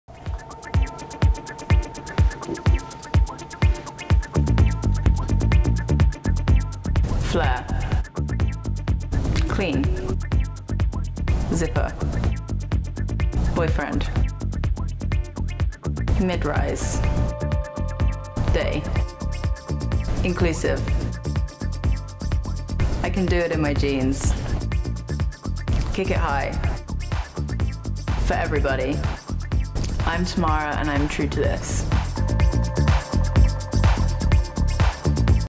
Tamara Bell - Volcom For Every Body Muse | Volcom Women's
Volcom enlisted model Tamara Bell for our Volcom For Every Body denim campaign which embraces body positivity and features all-inclusive women's sizing.